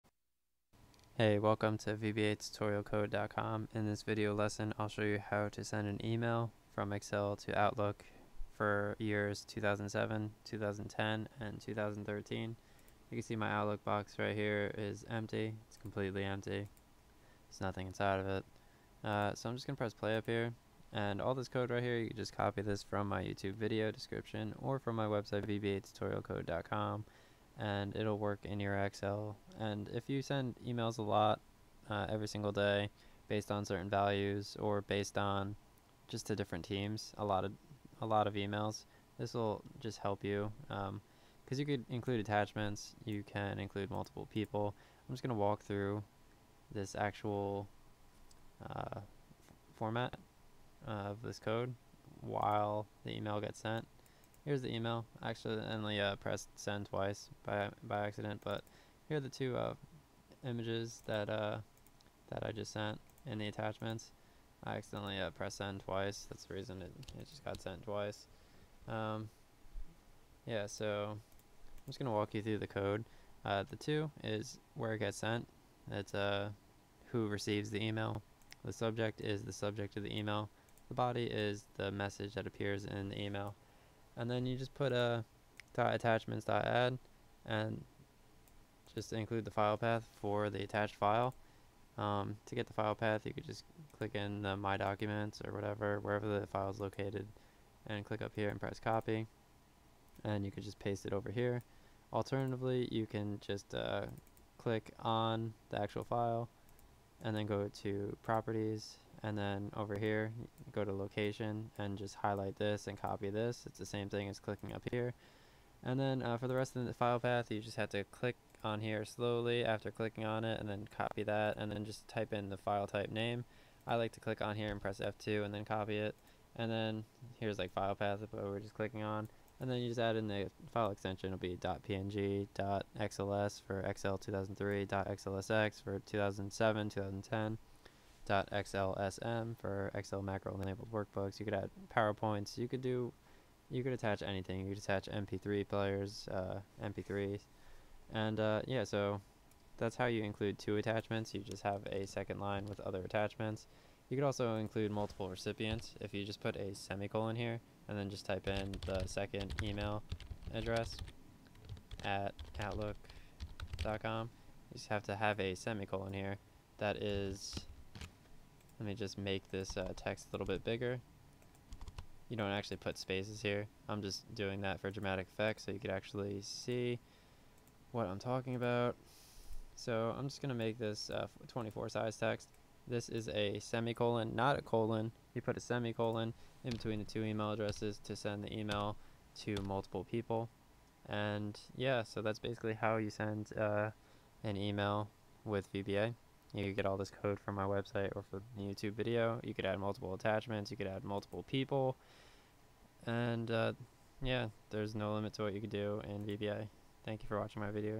VBA Email Excel Multiple Attachments and People in Outlook
VBA Email Excel. This works with Excel and Outlook 2007, 2010, and 2013. This video shows you how to send emails to multiple recipients. This video also shows you how to send emails with multiple attachments.

Source code below:
Sub EmailMacro()

Dim outapp As Object
Dim outmail As Object

Set outapp = CreateObject("Outlook.application")
Set outmail = outapp.createitem(0)

With outmail
.cc = ""
.bcc = ""
.Subject = "Test"
.Body = "Here is the message that appears in the body of the email."
.Attachments.Add ("C:\Users\Nonaluuluu\Desktop\Attachement1.png")
.Send
End With
On Error GoTo 0
Set outmail = Nothing
Set outapp = Nothing

End Sub